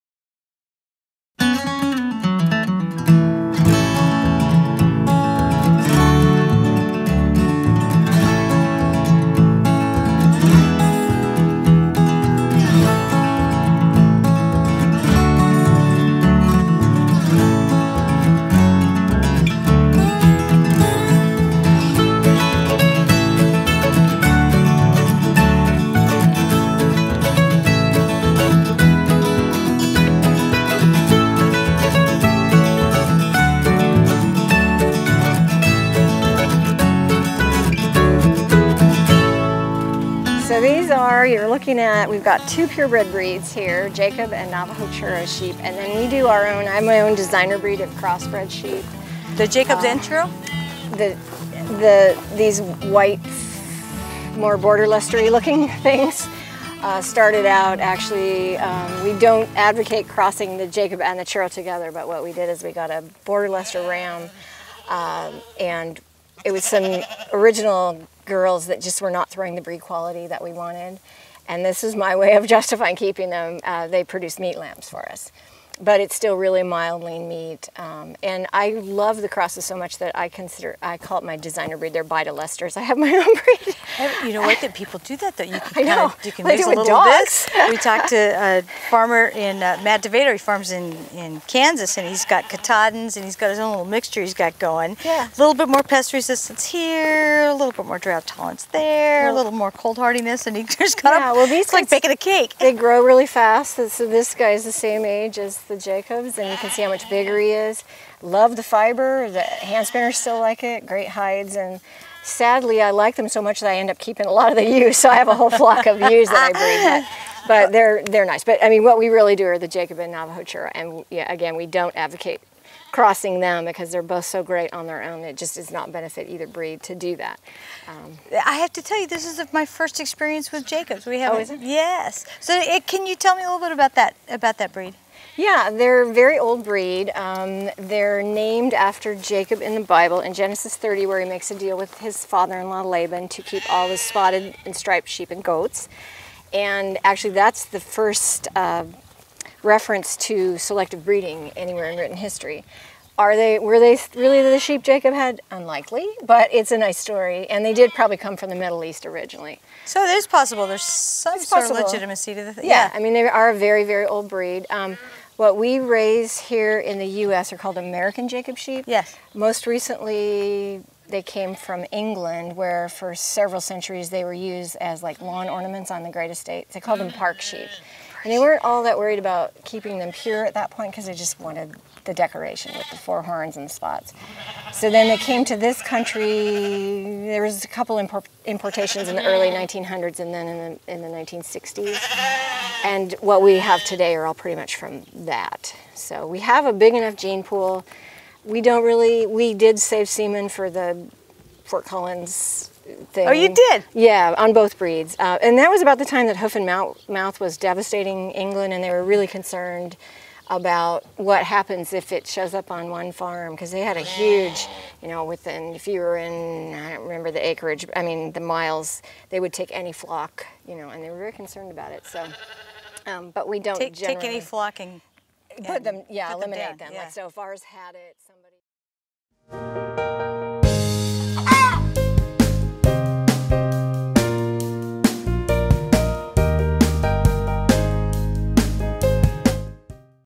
Bide a Wee Farm
While recently in the great state of Oregon, we had the pleasure to interview Karen of Bide a Wee Farm. She and her husband raise Jacob and Navajo Churro sheep in the town of Newberg. This is a short video from the interview. If you'd like to listen to the whole interview please go to hear it and others. to learn more about the Jacob and Navajo Churro sheep that Karen raises.